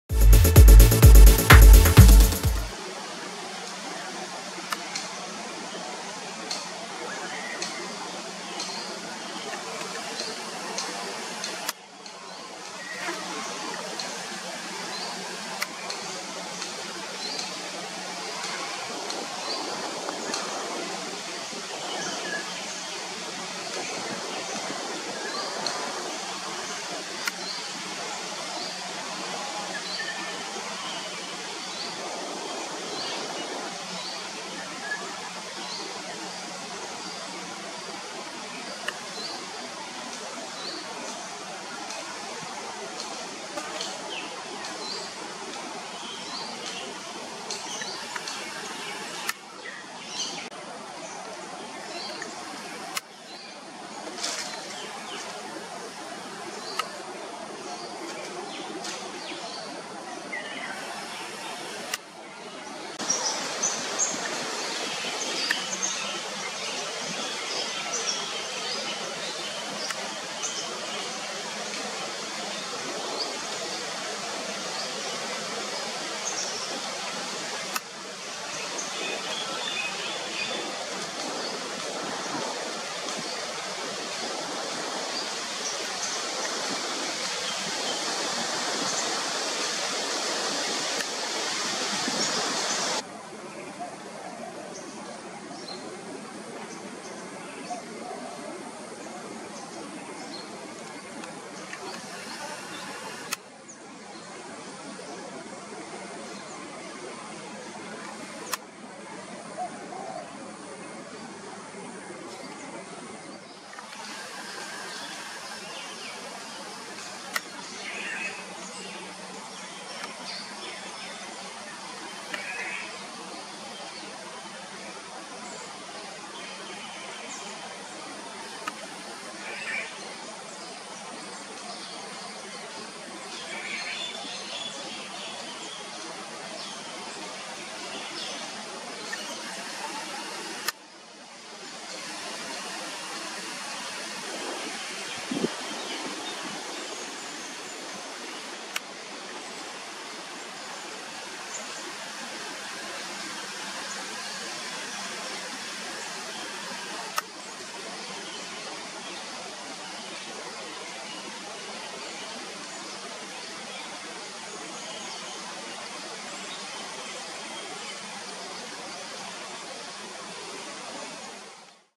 Chinese pond heron | Singapore | Bird Photography | Bird Watching | Jurong Lake Gardens
The Chinese pond heron is an East Asian freshwater bird of the heron family. It is one of six species of birds known as "pond herons". It is parapatric with the Indian pond heron to the west and the Javan pond heron to the south, and these three are presumed to form a superspecies.